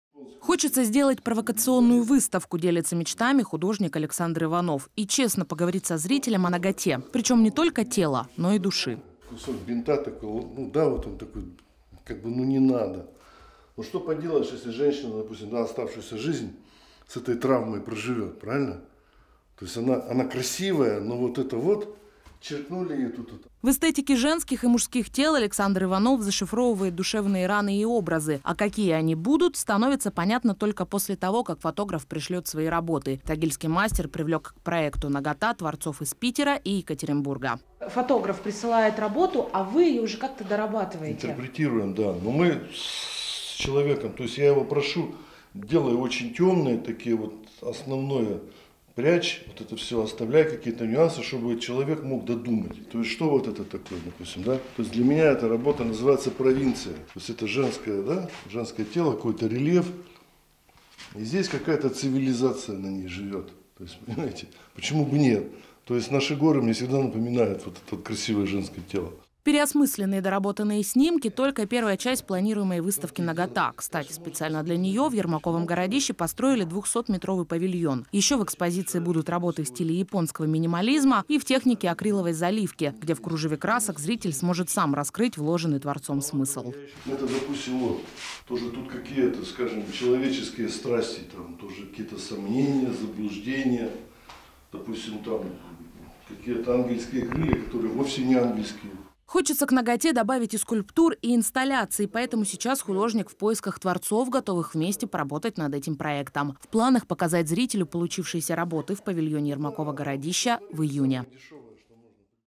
Нагота тела и не только
Переосмысленная "Нагота". И это даже не столько про тело. Доработанные снимки своих коллег по цеху тагильский скульптор, член союза художников России Александр Иванов готовит для выставки про наготу. Она, по его мнению, зашифровывает душевные раны и образы.